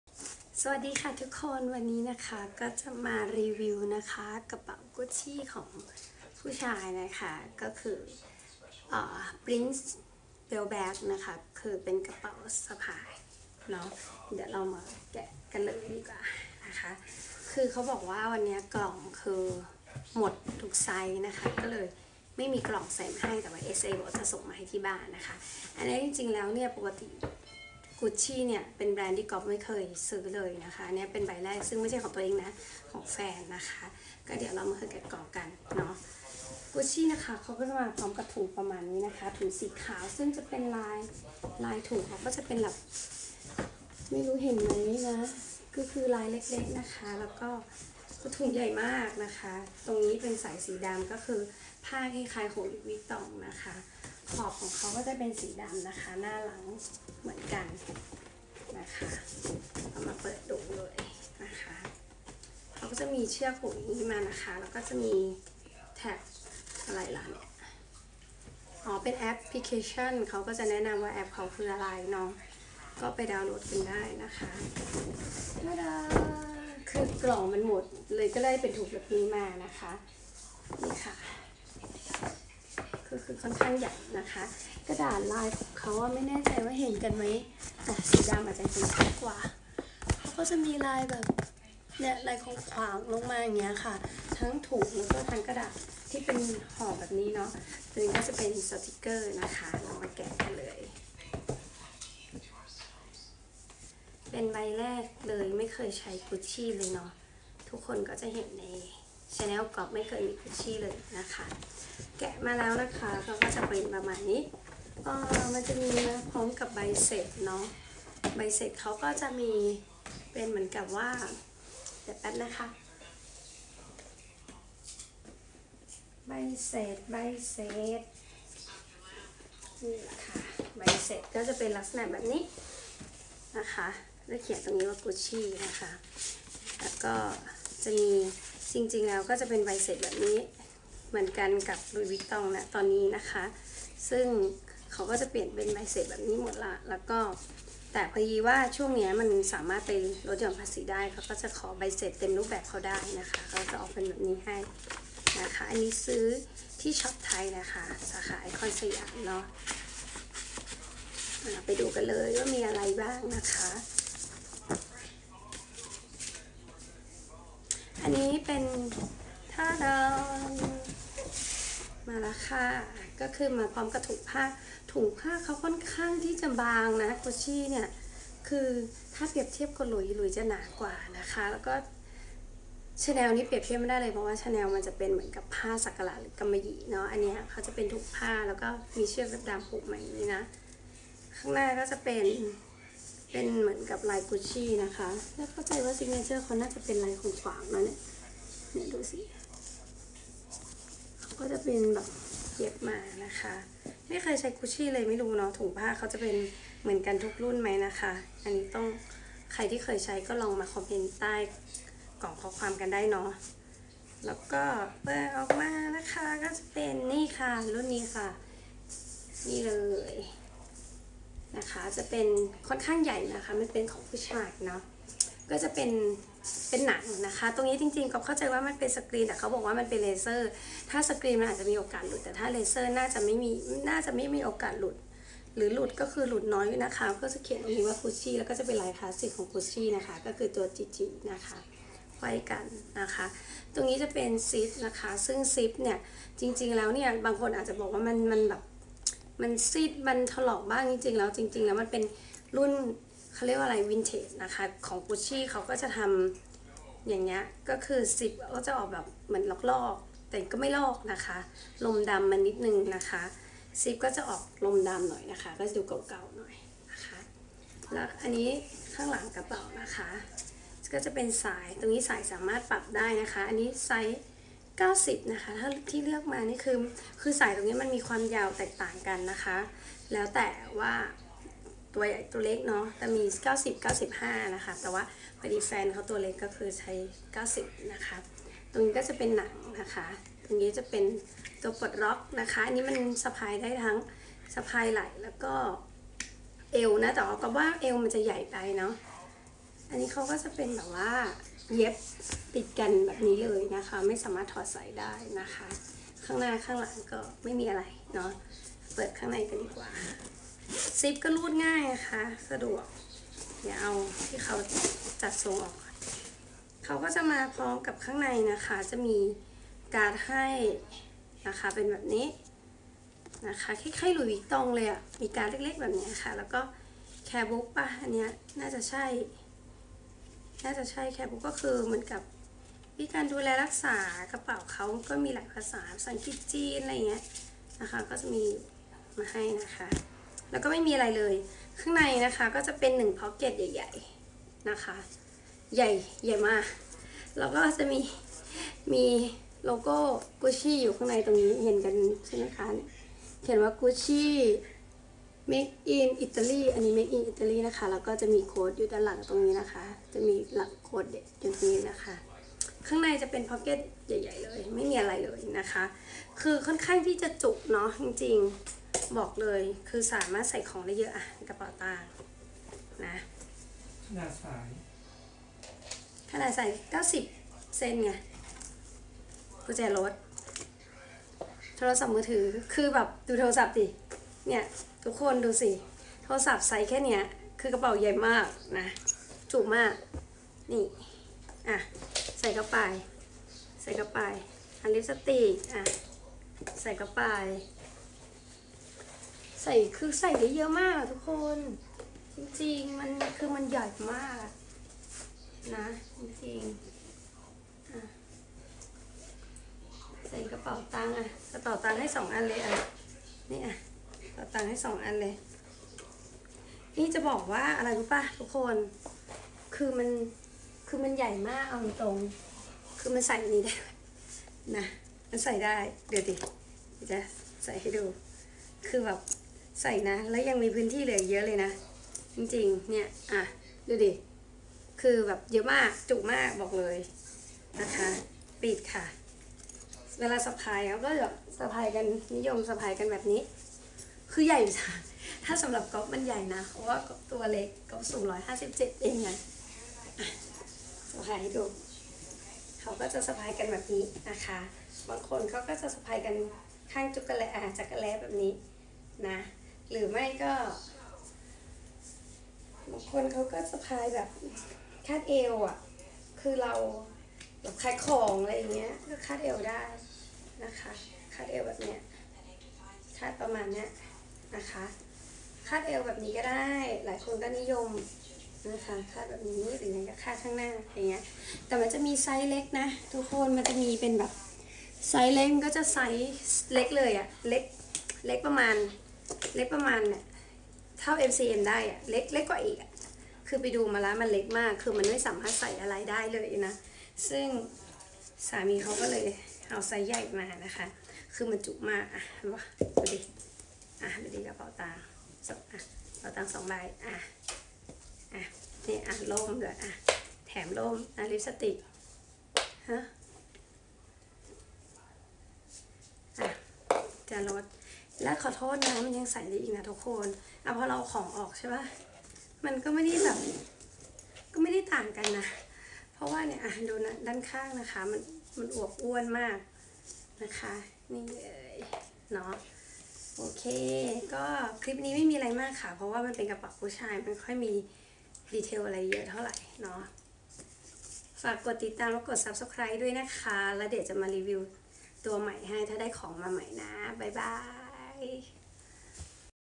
รีวิวกระเป๋า Gucci Print leather belt bag men
รีวิวกระเป๋า Gucci Print leather belt bag men กระเป๋ากู้ดชี่คาดเอวได้ คาดอก เป็นไซส์ใหญ่ สาย90เซน ของผู้ชาย ช้อปในไทย